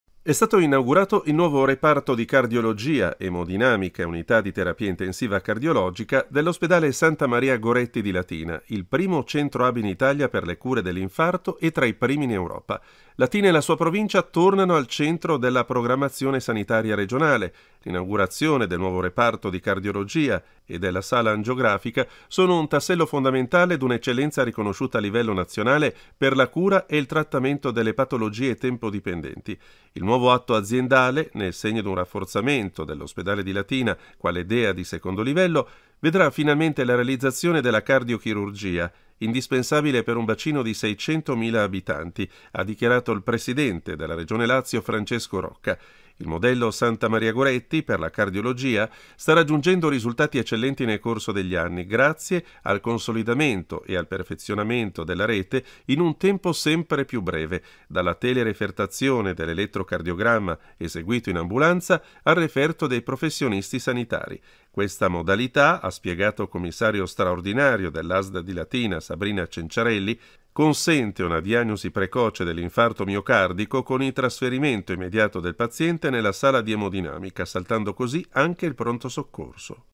Inaugurato nuovo reparto cardiologia al Goretti
E’ stato inaugurato il nuovo reparto di Cardiologia, Emodinamica e Unità di terapia intensiva cardiologica dell'ospedale Santa Maria Goretti di Latina, il primo centro hub in Italia per le cure dell'infarto e tra i primi in Europa.

"Latina e la sua provincia tornano al centro della programmazione sanitaria regionale. L'inaugurazione del nuovo reparto di Cardiologia e della Sala Angiografica sono un tassello fondamentale di un'eccellenza riconosciuta a livello nazionale per la cura e il trattamento delle patologie tempo-dipendenti. Il nuovo atto aziendale, nel segno di un rafforzamento dell'Ospedale di Latina quale Dea di II Livello, vedrà finalmente la realizzazione della cardiochirurgia, indispensabile per un bacino di 600mila abitanti" ha dichiarato il presidente della Regione Lazio Francesco Rocca. "Il modello Santa Maria Goretti per la Cardiologia sta raggiungendo risultati eccellenti nel corso degli anni, grazie al consolidamento e al perfezionamento della rete in un tempo sempre più breve: dalla tele-refertazione dell'elettrocardiogramma eseguito in ambulanza al referto dei professionisti sanitari. Questa modalità consente una diagnosi precoce dell'infarto miocardico, con il trasferimento immediato del paziente nella sala di Emodinamica, saltando, così, anche il pronto soccorso", ha affermato il commissario straordinario dell'Asl di Latina Sabrina Cenciarelli.

DOVE SEGUIRCI

GUARDACI IN TV SUL DIGITALE TERRESTRE:

Radio Roma News LCN Regione Lazio canale 14 DTT